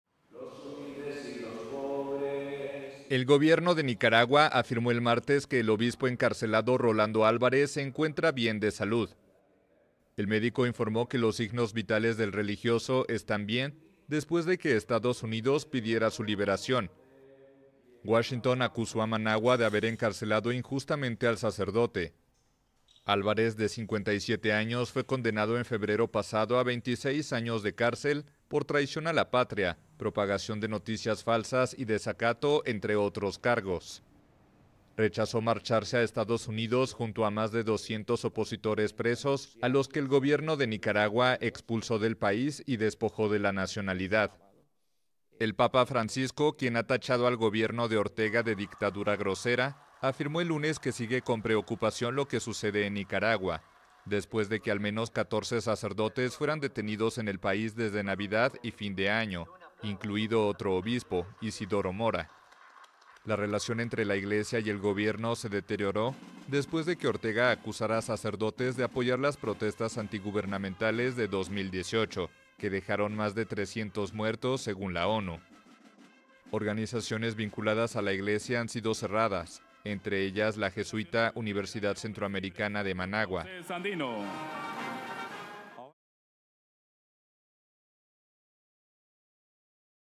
Gobierno de Nicaragua afirma que obispo preso está "bien" tras revisión médica
El gobierno nicaragüense aseguró que realizó una revisión médica al encarcelado obispo católico Rolando Álvarez, un crítico del presidente Daniel Ortega condenado a 26 años de prisión y cuya liberación urgió Estados Unidos el martes en medio de una ola de detenciones de sacerdotes en Nicaragua.